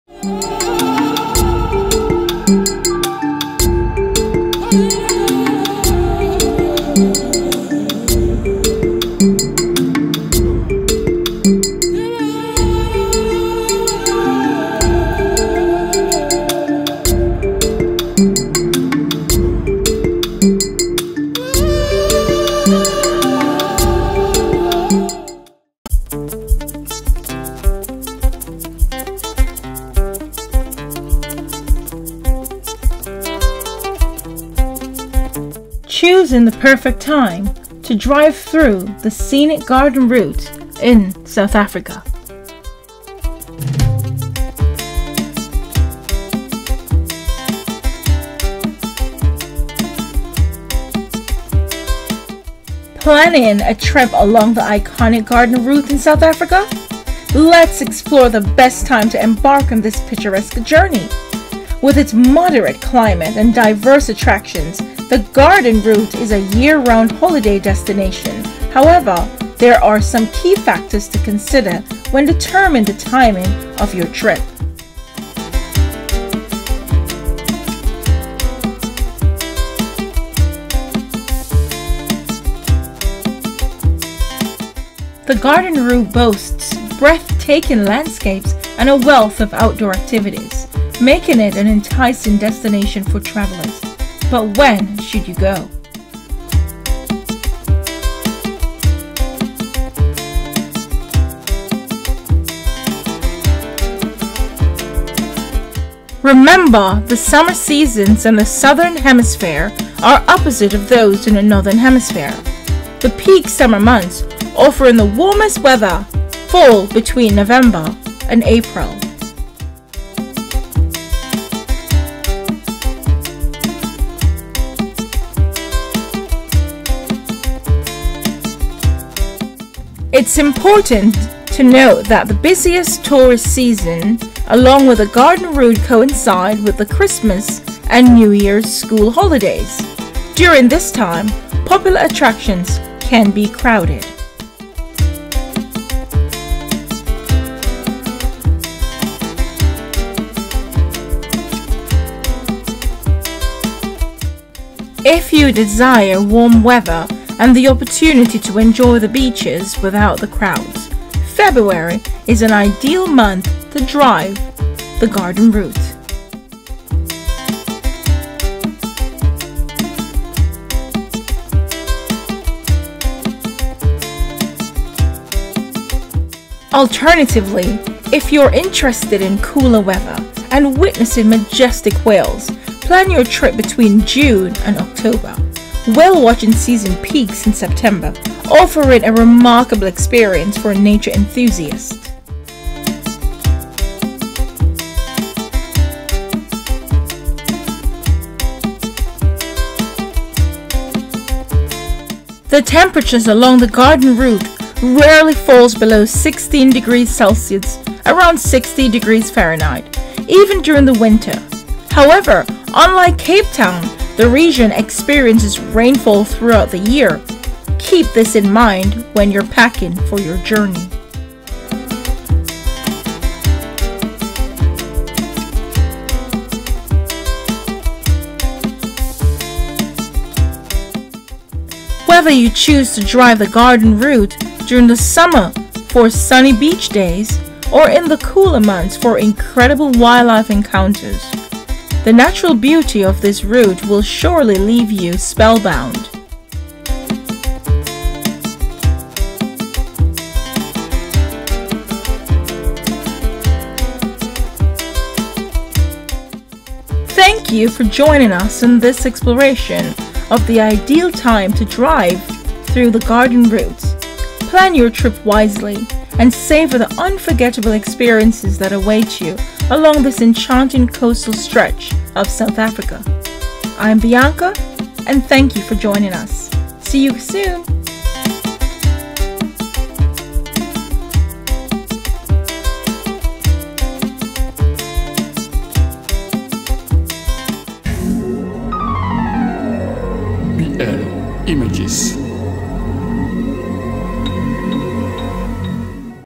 The Perfect Time To Drive The Garden Route In South Africa
The Garden Route in South Africa is a breathtakingly beautiful drive that offers stunning views of the coastline, mountains, and forests. But when is the best time to take this scenic drive? In this video, we'll explore the factors that can affect your experience on the Garden Route and help you choose the perfect time to take this unforgettable journey.